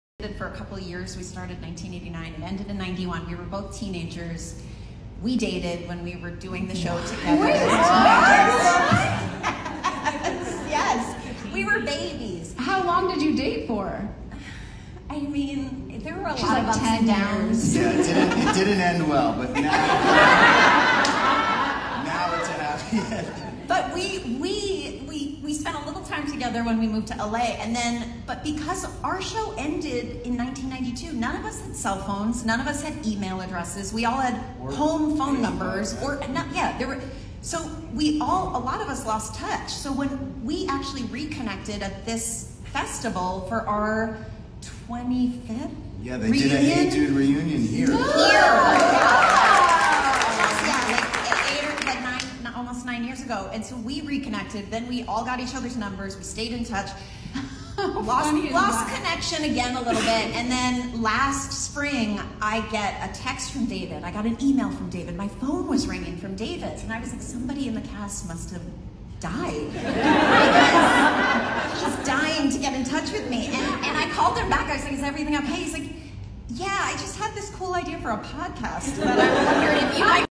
Hey Dude Stars Christine Taylor and David Lascher at ATX TV Festival
Christine Taylor talks about dating David Lascher during Hey Dude and how they started their podcast.